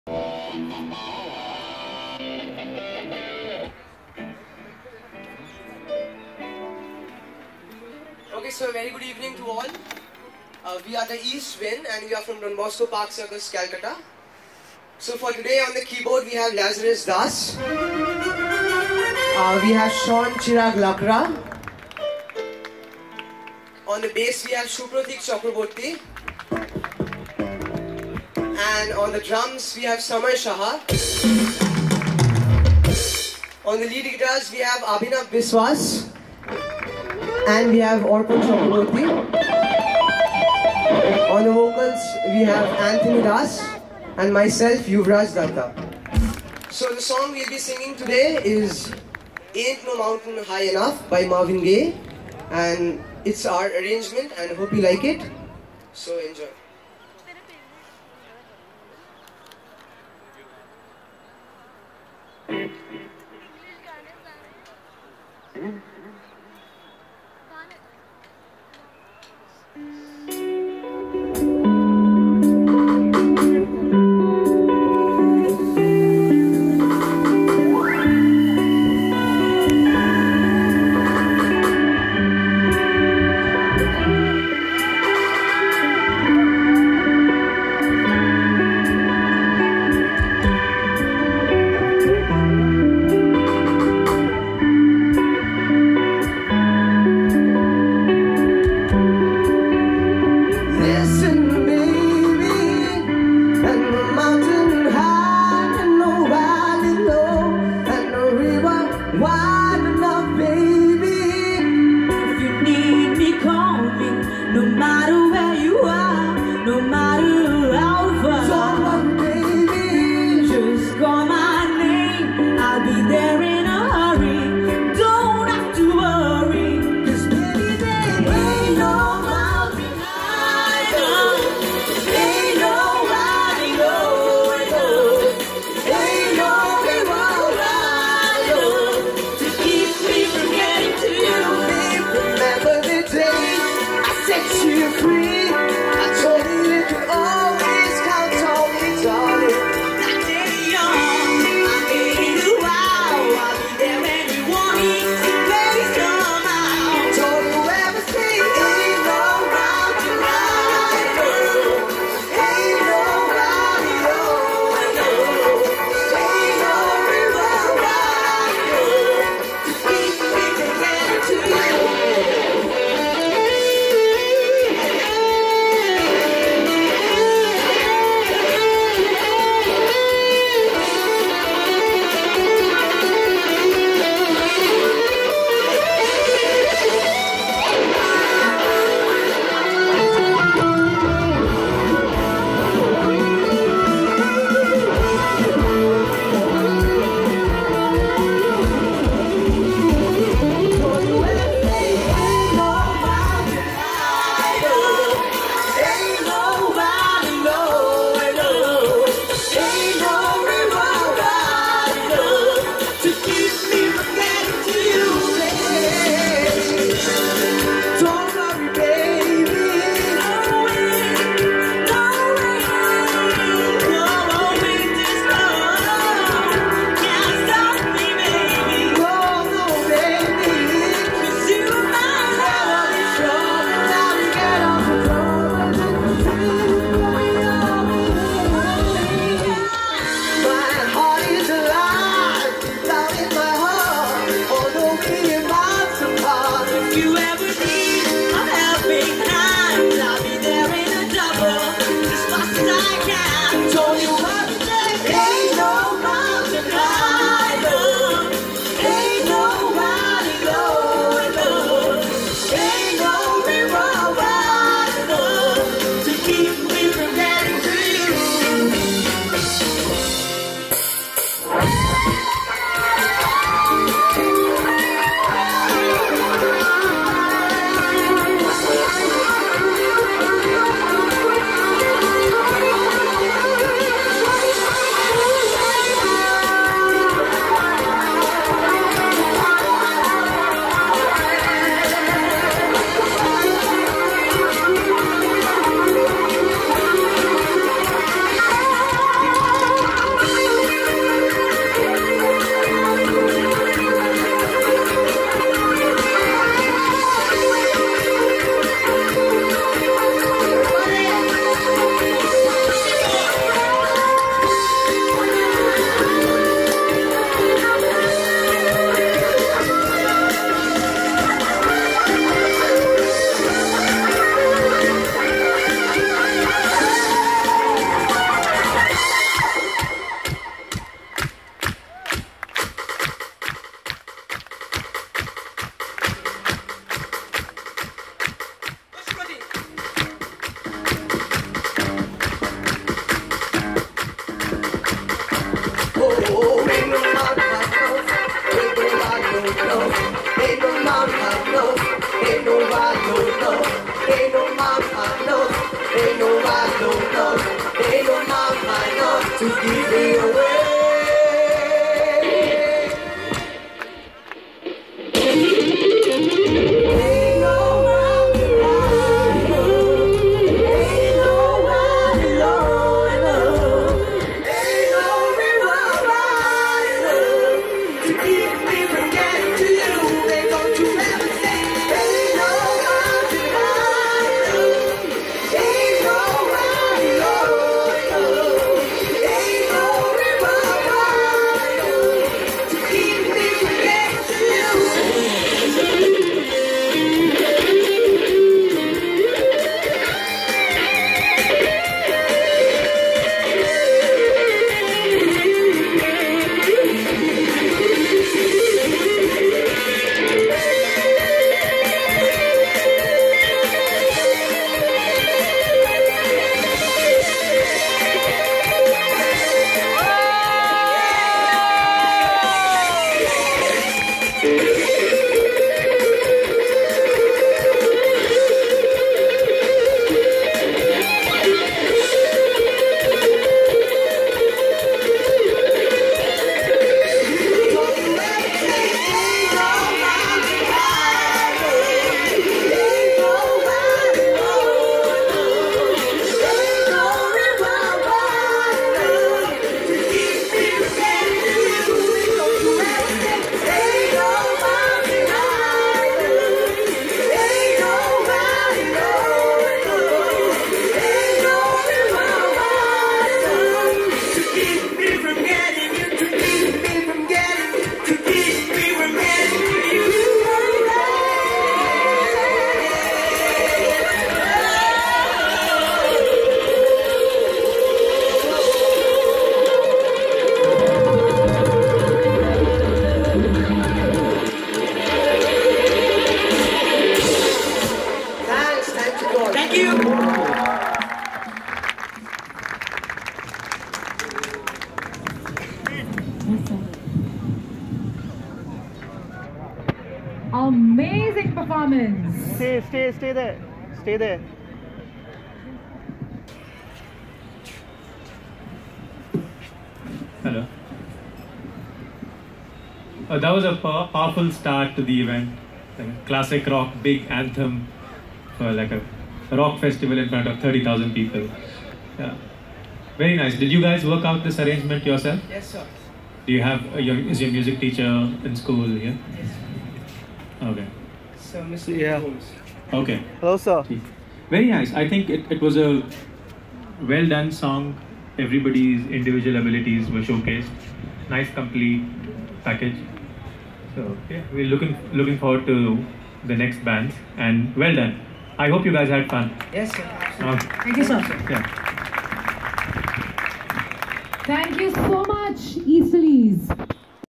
Yamaha Teens Rock - Don Bosco Park Circus @Delhi Finals Sep 2018
DBPC Kolkata School Band Performance in Delhi - Finals on 23 Sep 2018